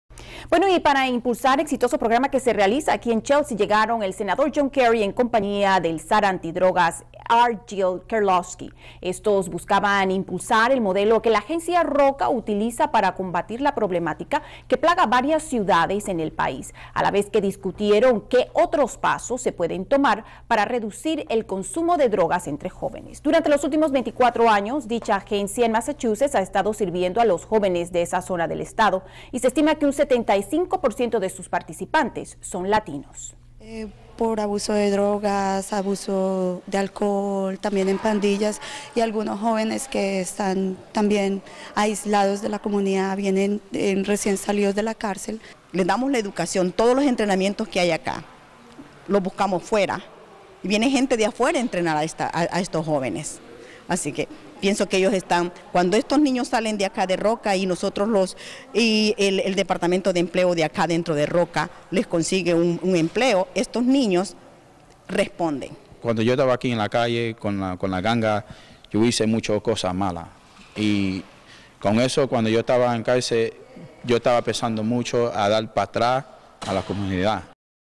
Combatiendo las Drogas
Exitoso programa que se realiza en Chelsea llegaron el senador John Kerry en compañia del Zar anti- drogas, R. Gil Kerli-kowske.